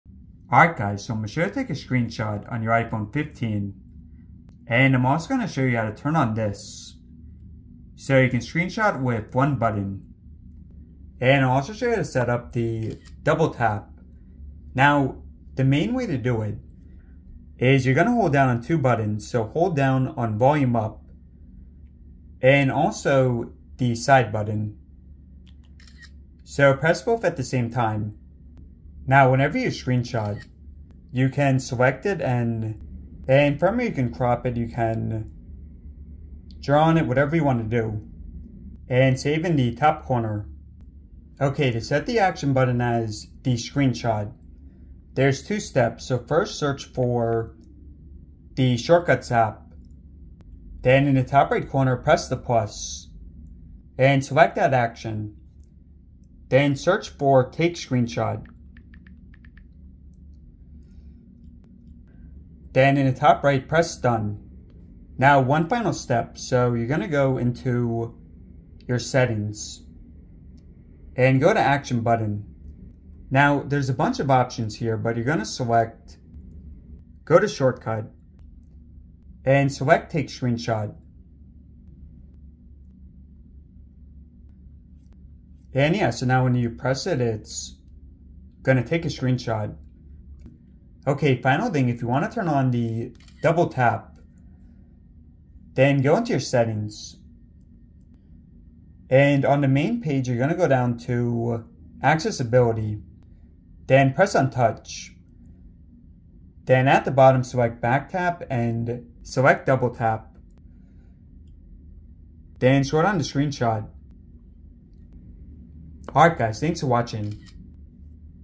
iPhone 15 / 15 Pro / Plus - How to Screenshot!
How to take a screenshot on iPhone 15, 15 Pro / Pro Max, or 15 Plus. Also how to set Action Button to screenshot and turn on Double Tap to screenshot.